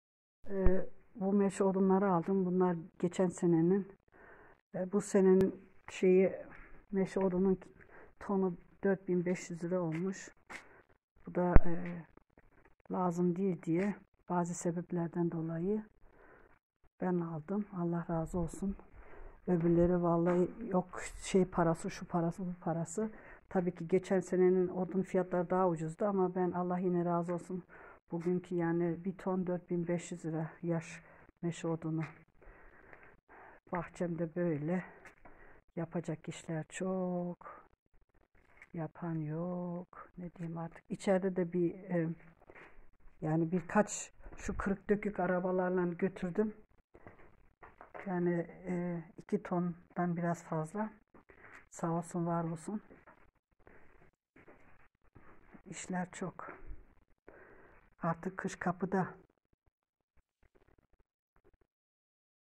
Bunlar geçen seneden kalan odunlar.Bugünkü sade yaş meşe 1t 4500.Kesme,getir,götür bilmem ne hariç.
Kış kapıda,geçen seneden kalma meşe odunları...